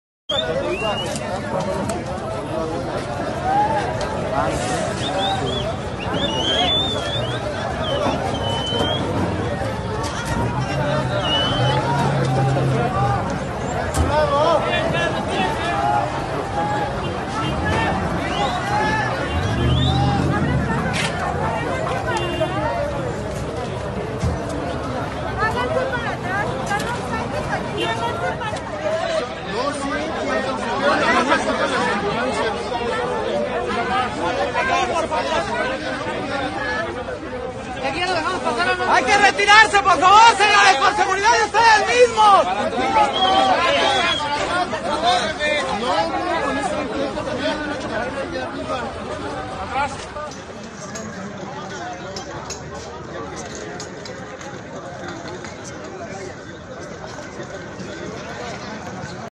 💥 Explosión de Gas DESTRUYE vecindad en la Col. Morelos 🔴
17 lesionados y 3 departamentos colapsados tras explosión de cilindro de 20 kg por fuga de gas LP al interior de un domicilio ubicado sobre las calles Carpintería y Panaderos en la Col. Morelos en la Alcaldía Venustiano Carranza.